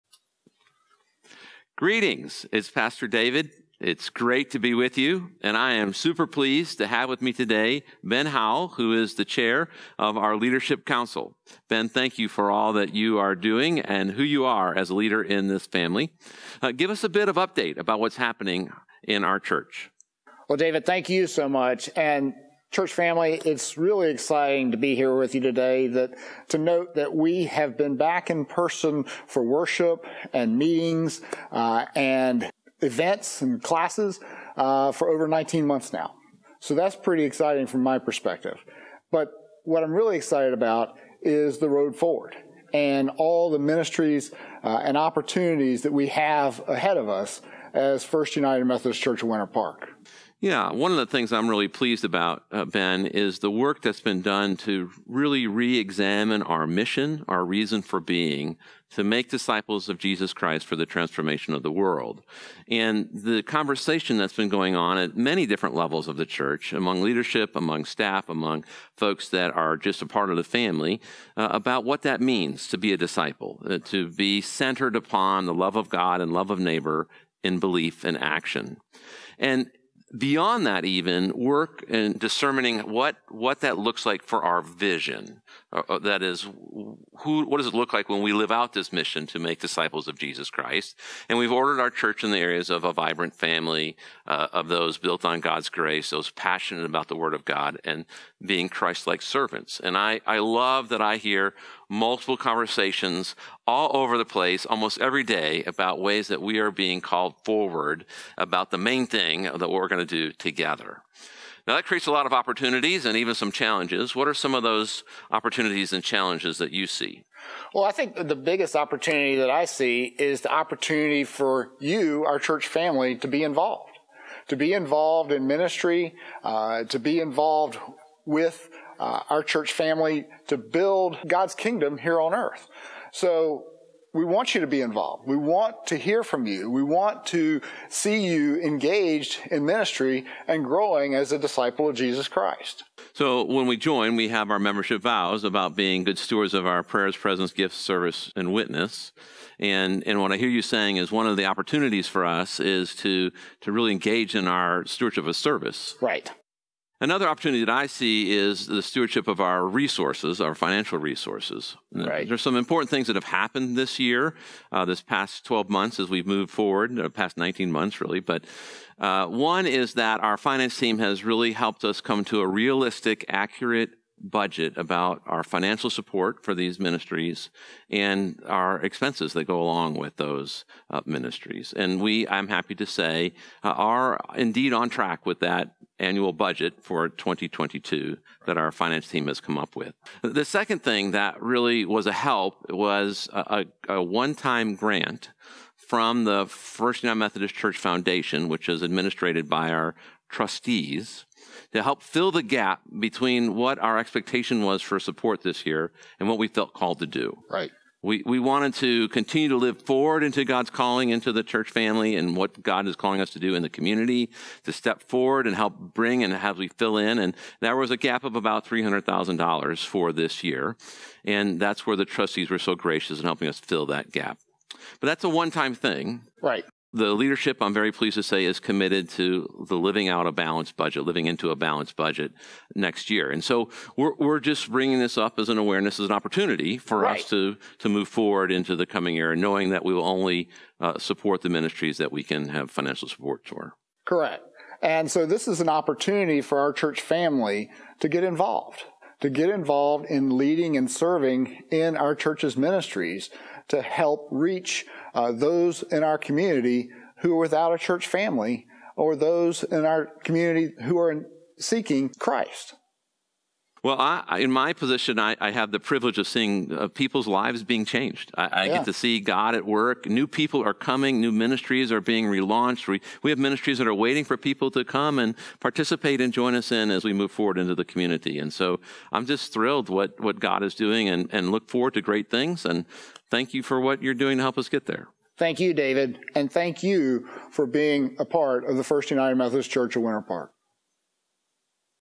Church & Stewardship Update from Pastor David & Ben Howell!
Check out this update to see where we're at as a church family and what's to come! We want you to get involved.